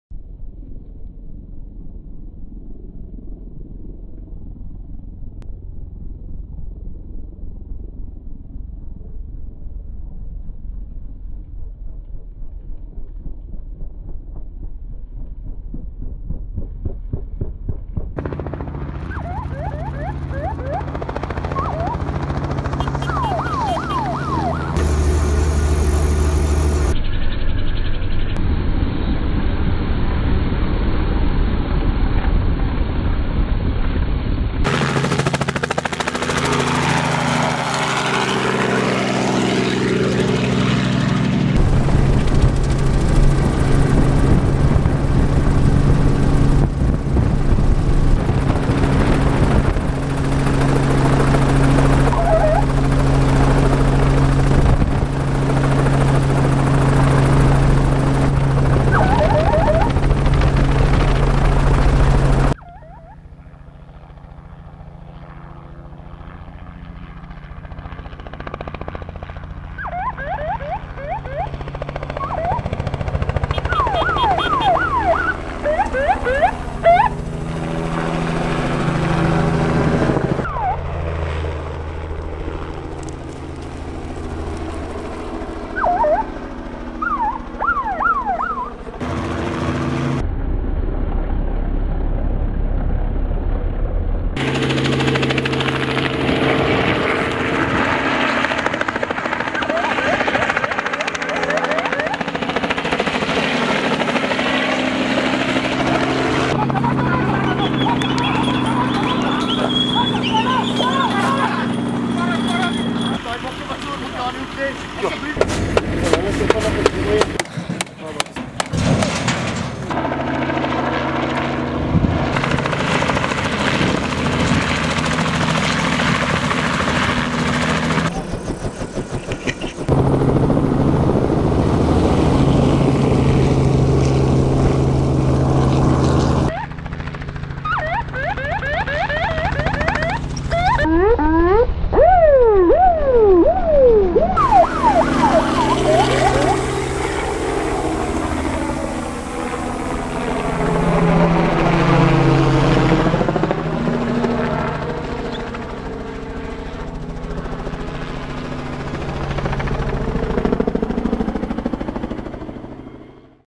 Crazy Game Catching South Africa
Moving game in South Africa is a very difficult task so here is a short video I made to show one way to do it.
These animals is in no way harmed, they are saved because of a massive drought in this Part of South Africa.
They are safely moved to a location where they have food and water.